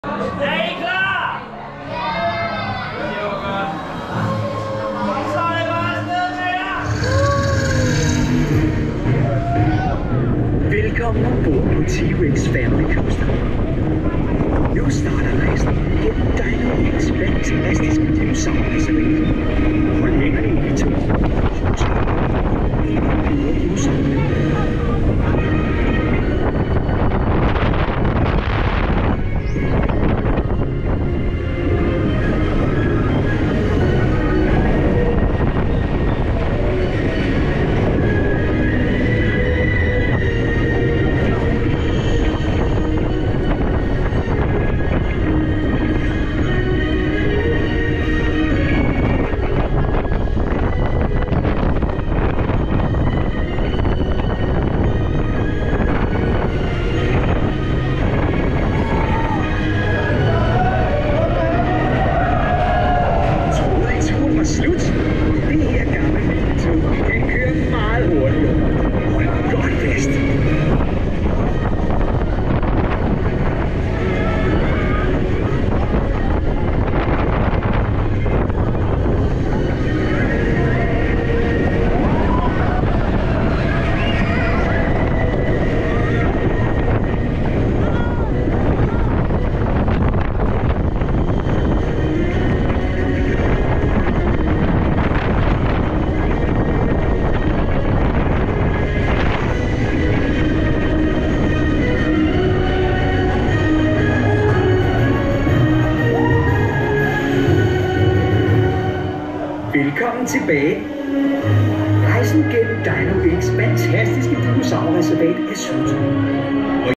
T-Rex Family Coaster POV (4K 60FPS), Djurs Sommerland New Mack Powered Coaster | Non-Copyright
T-Rex Family Coaster is the new-for-2022 Mack powered coaster at Djurs Sommerland.  This junior coaster is themed to dinosaurs as you wrap past multiple statues throughout the course.